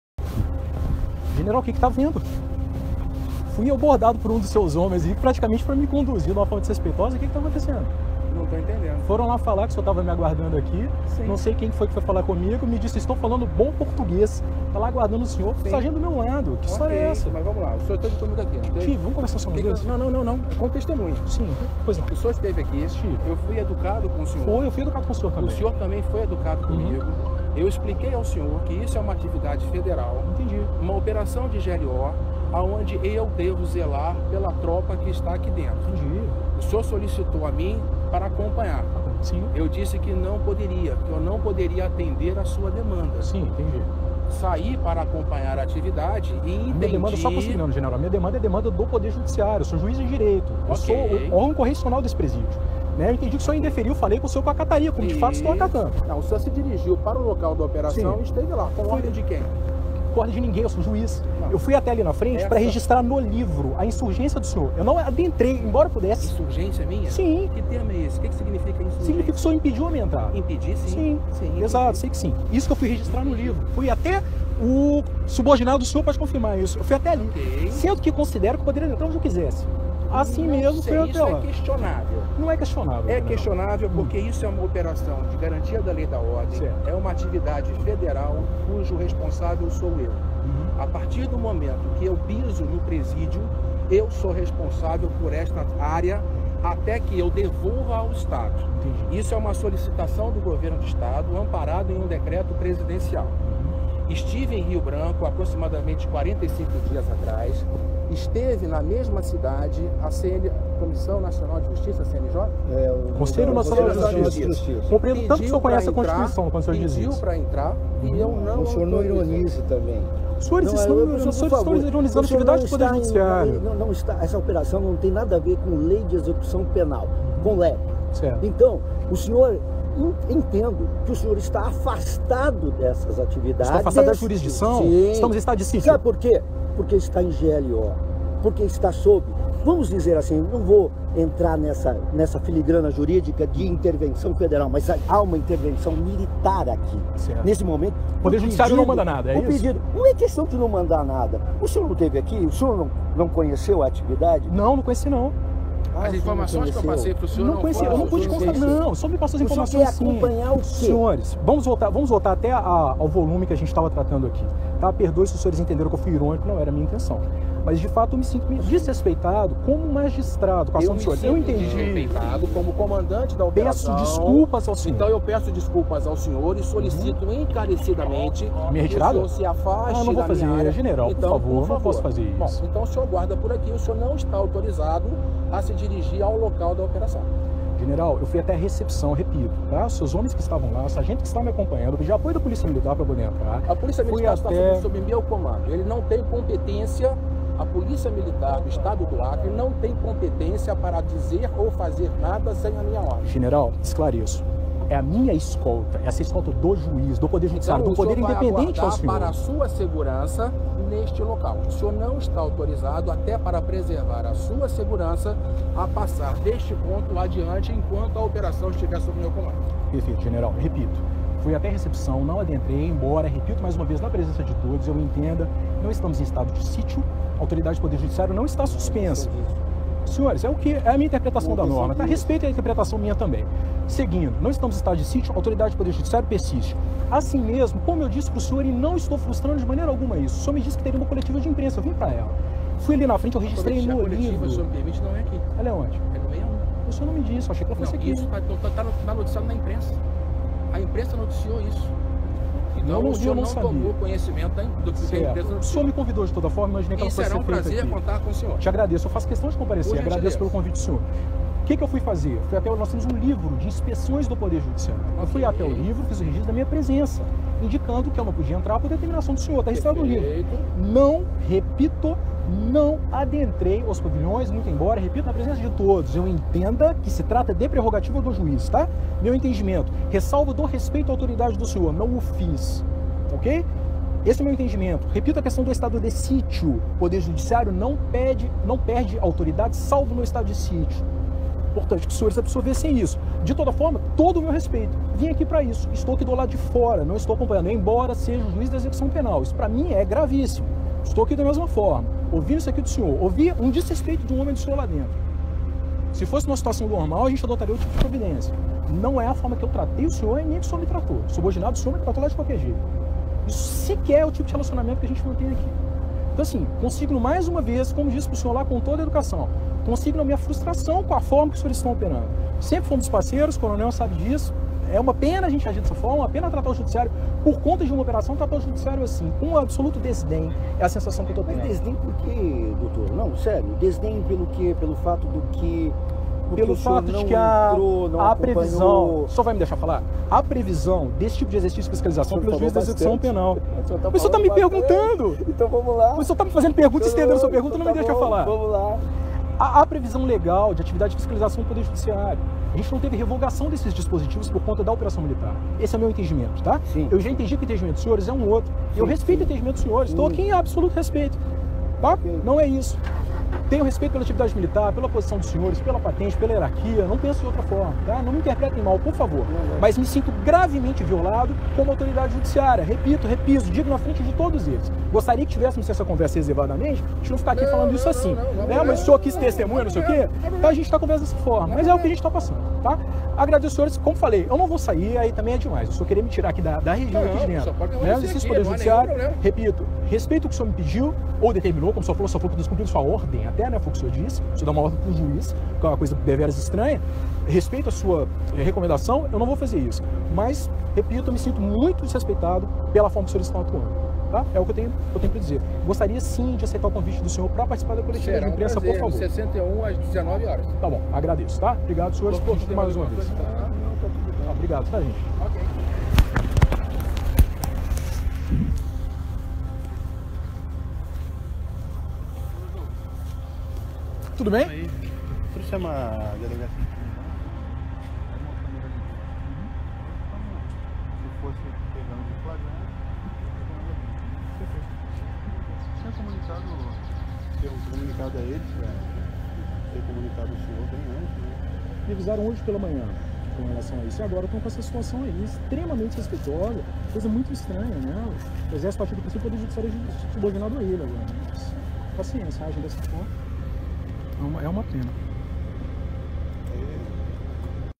General barra entrada de juiz em presídio no Acre
General barra entrada de juiz durante operação em presídio no Acre.

Em áudio divulgado pela Associação Nacional dos Magistrados Estaduais (Anamages) mostra o diálogo, em tom alterado, entre o general José Eduardo Leal de Oliveira, comandante da 17ª Brigada de infantaria de Selva, e o juiz titular da Vara de Execuções Penais de Cruzeiro do Sul, Hugo Torquato. O juiz foi impedido de acompanhar a Operação Thaumaturgo, que fez uma varredura no presídio Manoel Neri, na última terça-feira (11).

O áudio começa no momento em que o juiz vai ao encontro do general questionar o motivo de ter sido barrado por um dos militares do Exército e recebe uma resposta atravessada. Ao G1, a assessoria do Exército informou que "tem o caso como encerrado".

“Essa é uma operação federal, o senhor esteve aqui, eu fui educado com o senhor e expliquei que essa é uma atividade federal, uma operação de GLO [Garantia da Lei e da Ordem] aonde eu devo zelar pela tropa que está aqui dentro. O senhor solicitou a mim para acompanhar e eu disse que o senhor não poderia acompanhar. O senhor se dirigiu ao local da operação com ordem de quem?”, questiona o general.

Diante da pergunta do general, o juiz responde: “Com ordem de ninguém, eu sou juiz. E o senhor impediu a minha entrada”.
O general, que está acompanhado de outras pessoas, continua afirmando que por se tratar de uma operação federal, o juiz não tem permissão para participar da ação.

"É uma atividade federal, cujo responsável sou eu. A partir do momento que eu piso no presídio, sou responsável por essa área até que eu devolva ao Estado. Isso é uma solicitação do governo do estado amparado em um decreto presidencial”, diz o general.
Um outro militar que acompanha a discussão, interrompe os dois e diz: “Essa operação não tem nada a ver com lei de execução penal. Entendo que o senhor está afastado dessa jurisdição. Há uma intervenção militar aqui. O senhor quer acompanhar o quê?”.
O juiz insiste e diz que não há impedimento legal para que ele deixe de acompanhar a operação.

“Não estamos em estado de sítio, a autoridade do Poder Judiciário não está suspensa. Isso pra mim é gravíssimo. É uma pena tratar o Judiciário assim, com absoluto desdém. Me sinto muito desrespeitado”, diz.

A Associação dos Magistrados do Acre (Amac) e a Associação Nacional dos Magistrados Estaduais lamentaram o episódio.
O presidente da Anamages considerou “inconcebível que o ocorrido represente a posição oficial do Exército brasileiro” e afirmou que Associação vai tomar as medidas cabíveis “para que haja um perfeito esclarecimento sobre a conduta do responsável pela ação”.